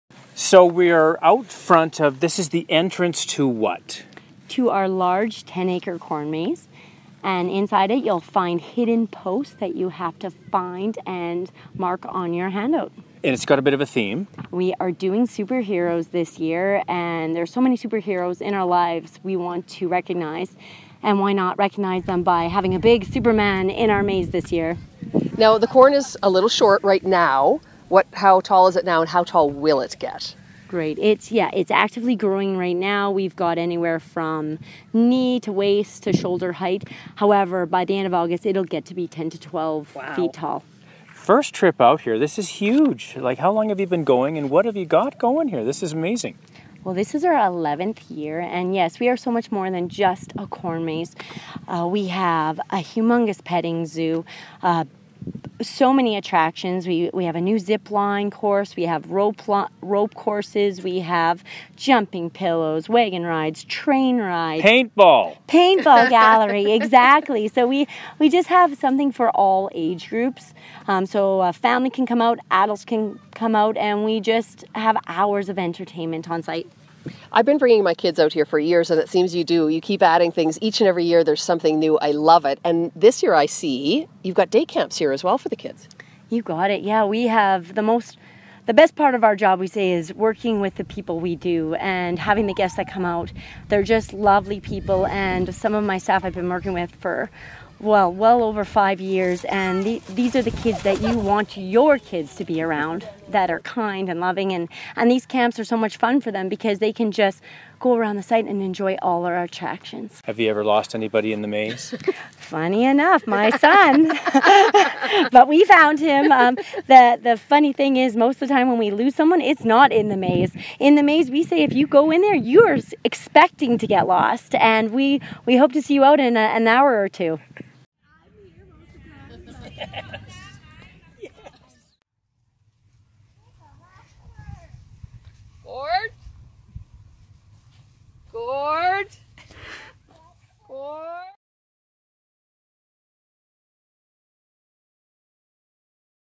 Superhero maze opens this weekend at the Calgary Corn Maze!
Can you believe that Gord Gillies had never been to the Calgary Corn Maze before!? Sue, Gord, and Sue's kids went along to the family fun farm this week for a little tour and sneak preview of this year's maze which opens July 28!